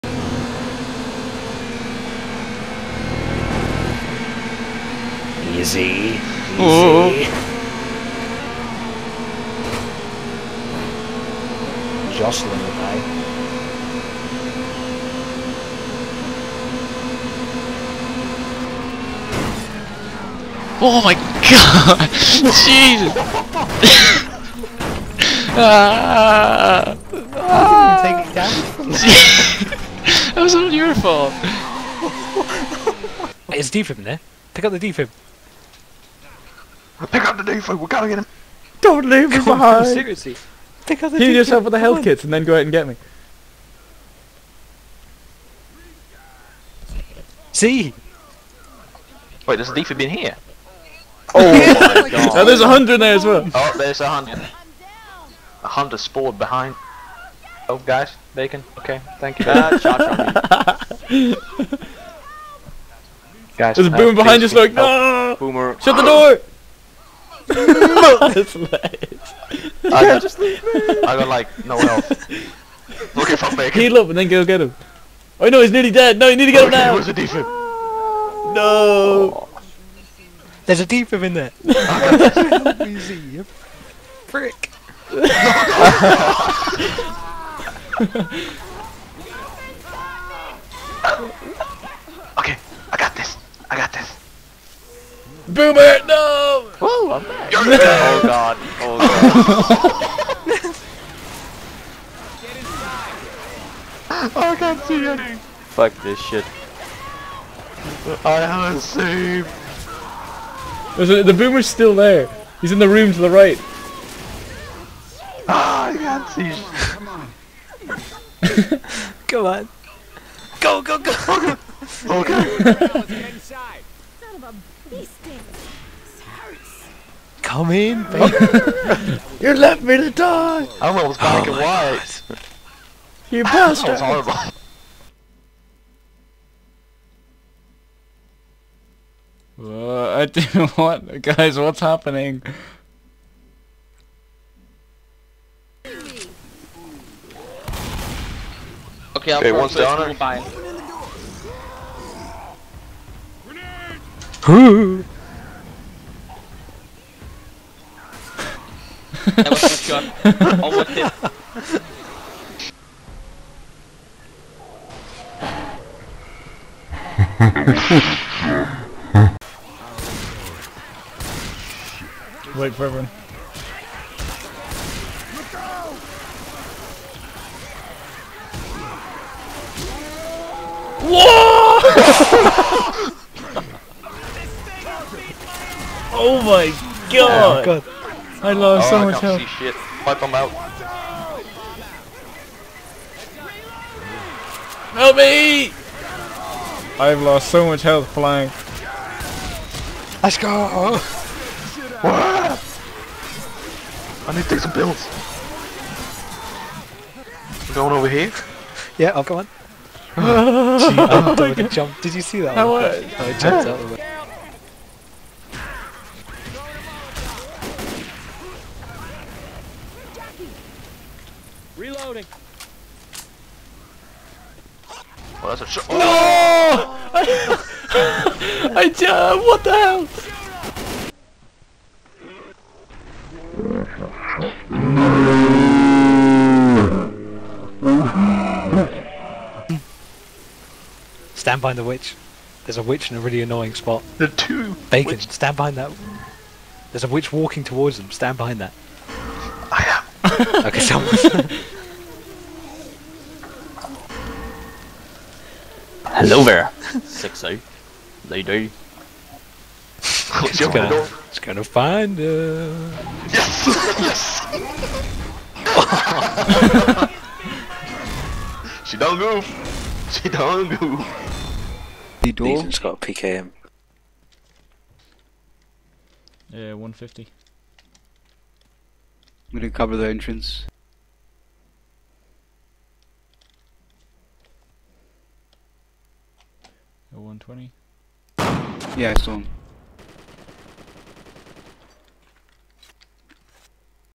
Pony Tales #2 - He dun goofed!
We tried our hand at a few different games this week, and as usual, we suck at everything. But it was kinda funny...